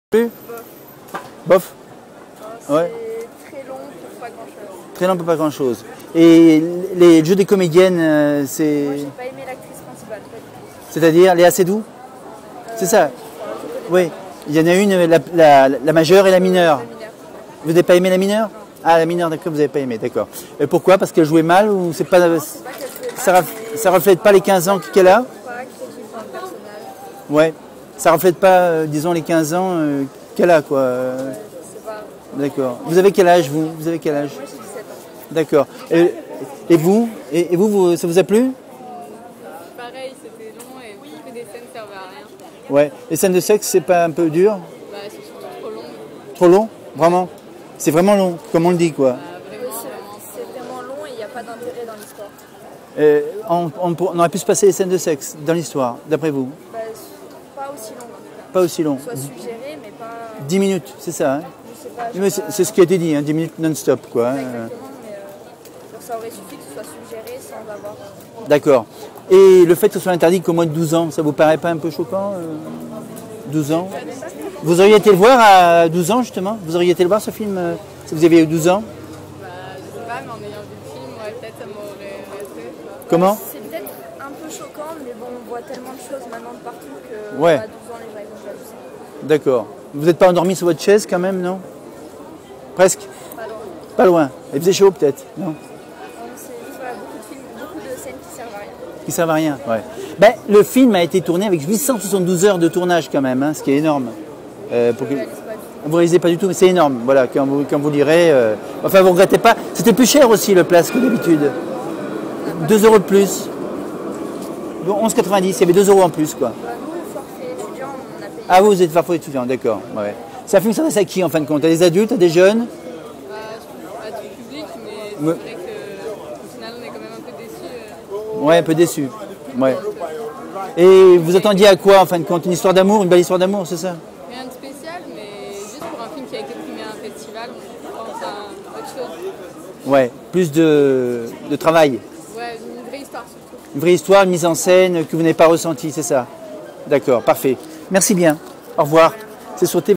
LA VIE D ADÈLE :" ON EST DÉÇU POUR UNE PALME !"
Un film trop long avec des scènes inutiles notamment les scènes de sex interdites qu aux moins de 12 ans .
Kechiche qui a été comédien avait interprète  un jeune beur objet de fantasme sexuel dans les innocents et fier gigolo de touristes occidentales ! Peut être une explication a cette débauche de scènes crues !